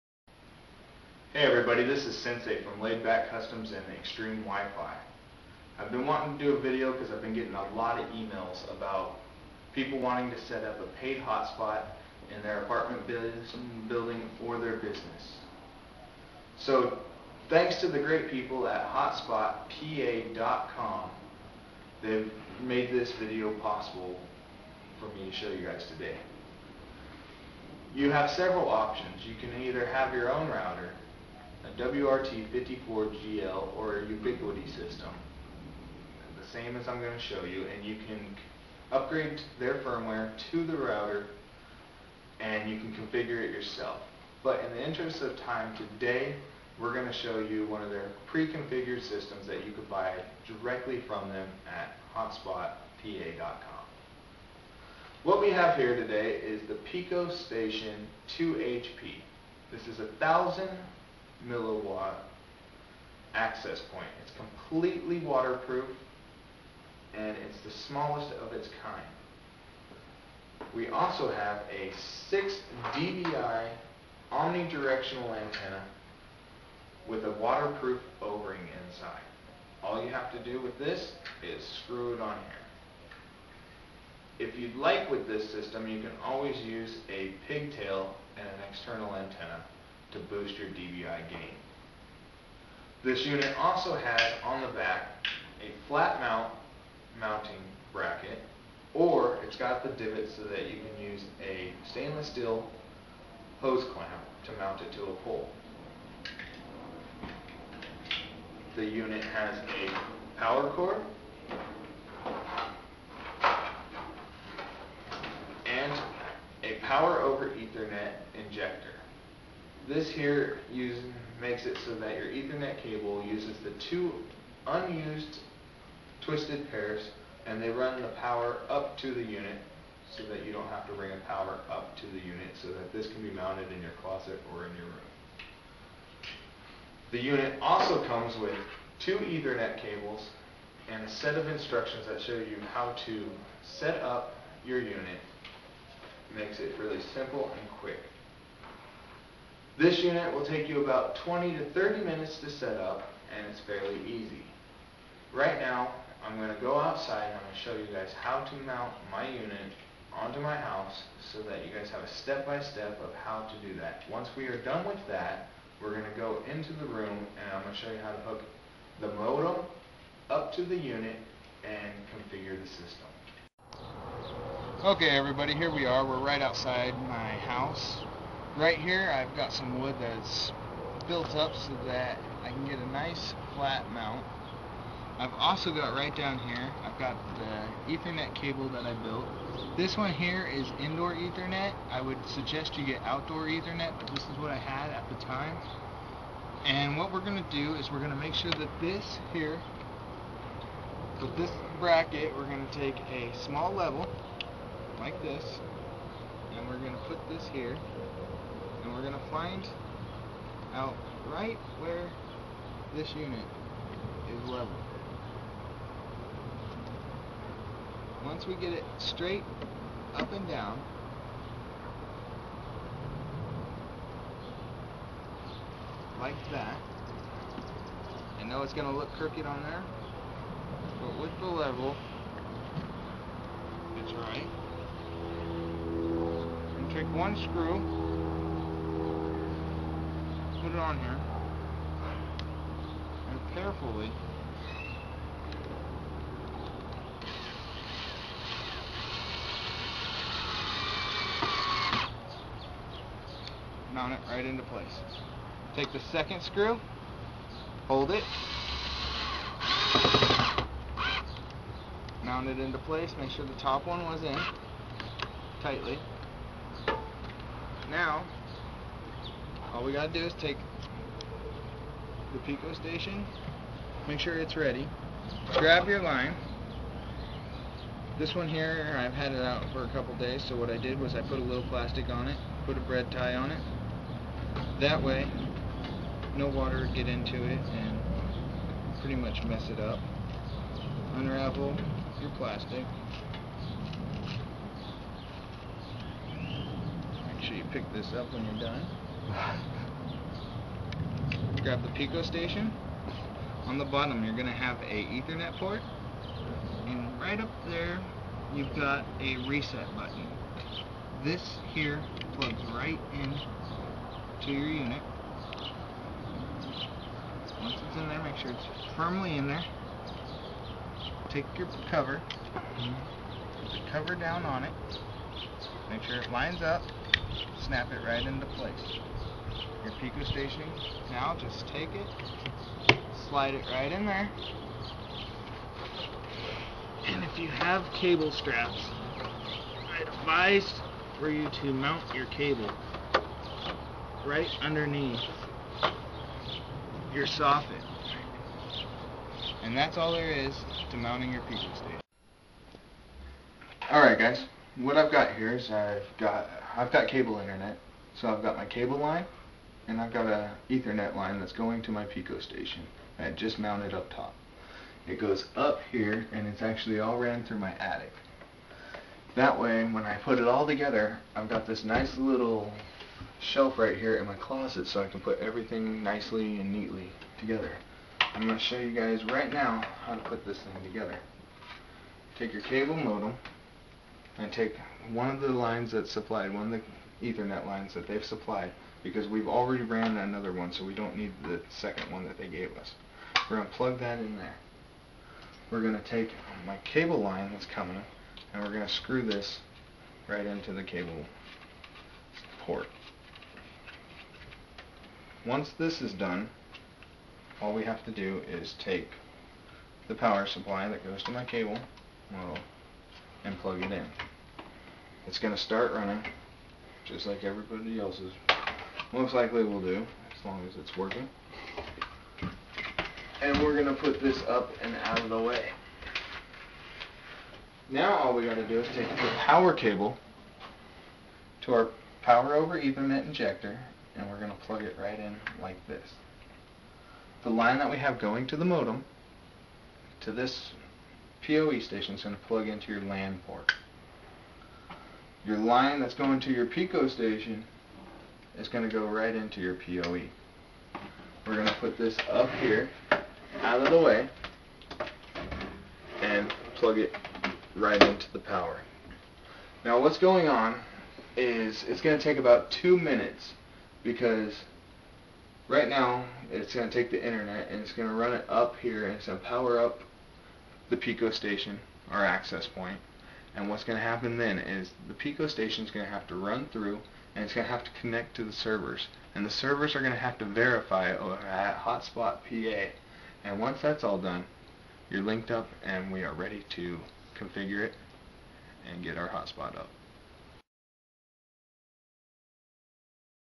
how to setup a wireless hotspot part 1
how to setup a wifi hotspot using hotspotpa.com's hotspot kit